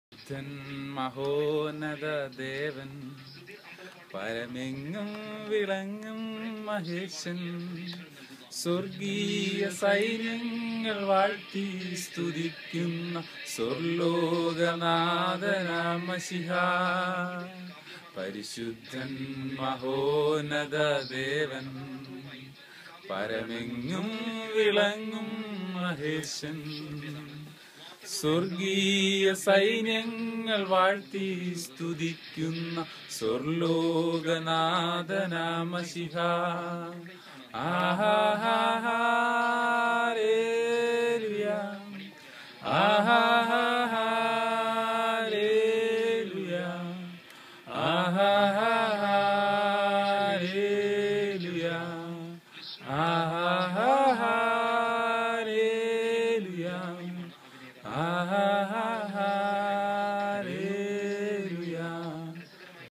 Repsol Rajan
Repsol and Ronald singing at MAHARANI NEW YORK